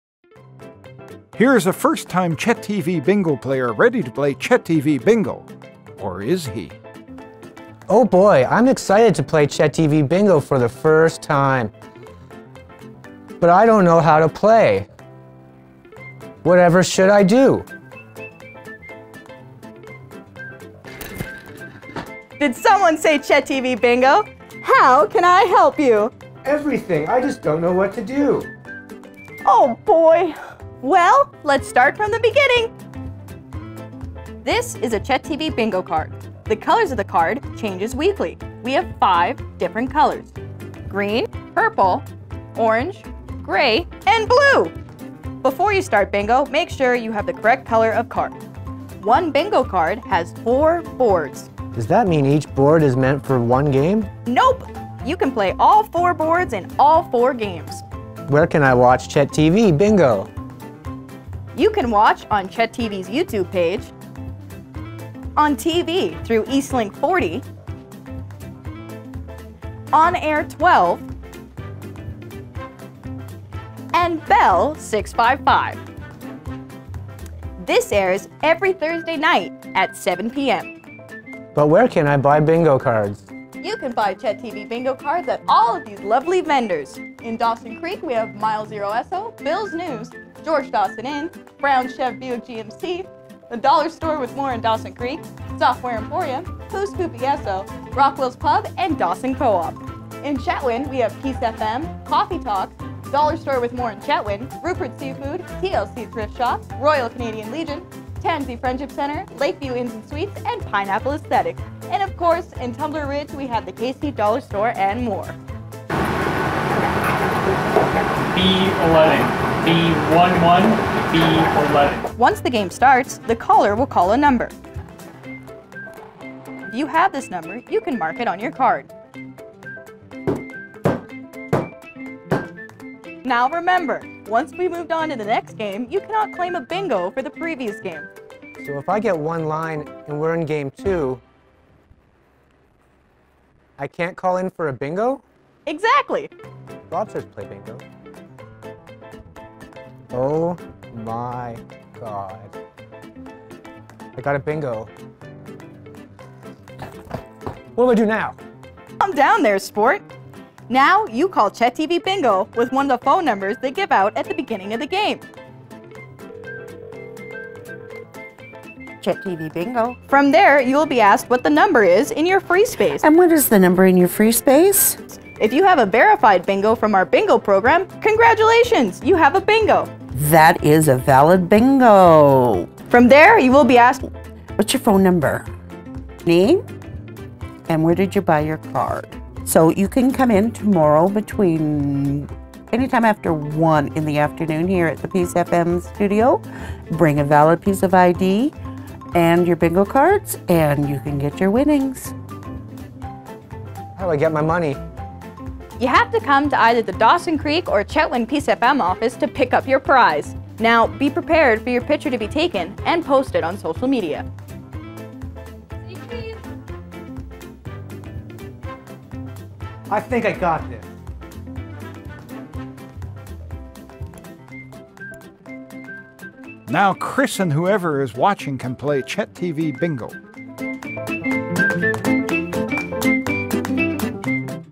CHET TV Bingo Tutorial
Is this your first time playing CHET TV Bingo? Here's a quick tutorial explaining the rules!